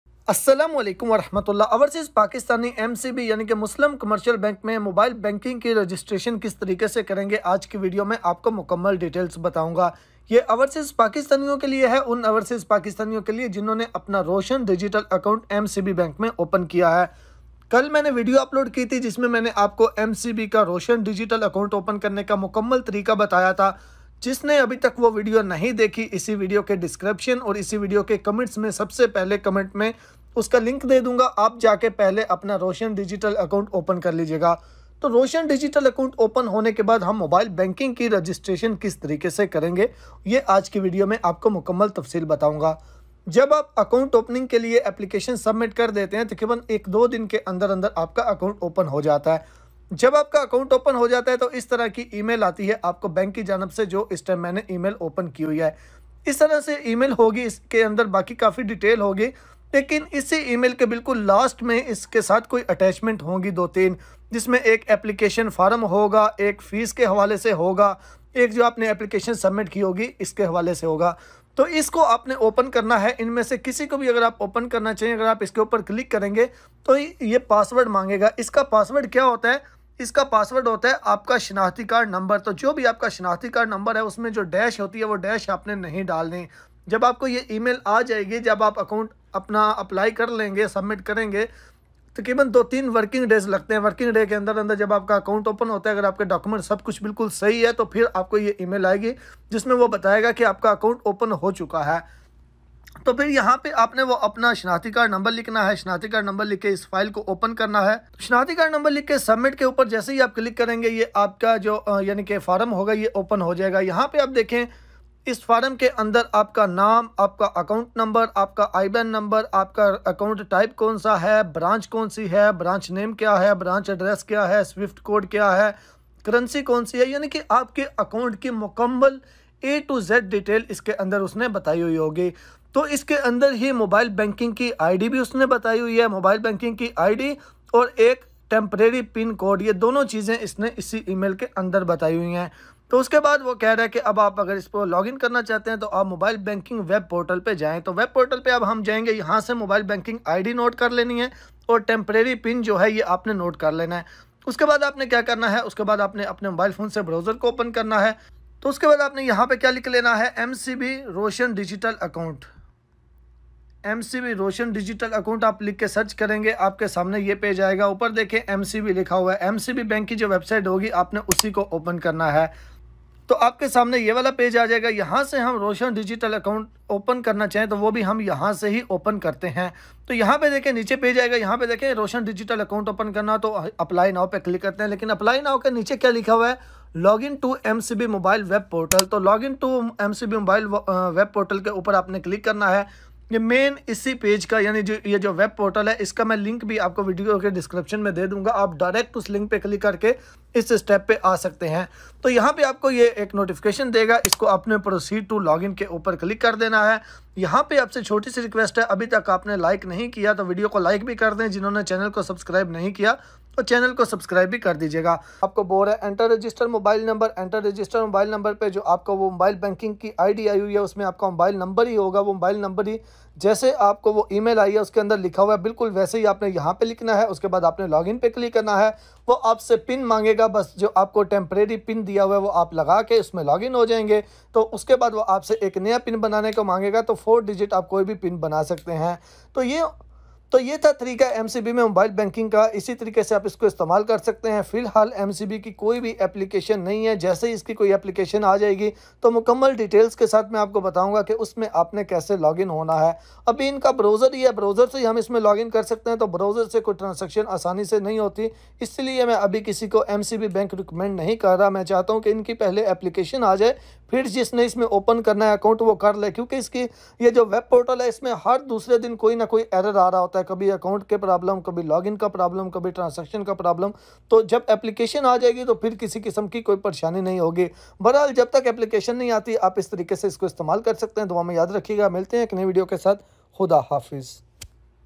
How to Register for MCB Mobile Banking | MCB Bank Mobile Banking Registration
MCB May mobile banking kaisy use kren gay aj ki video may details share krung apkai sath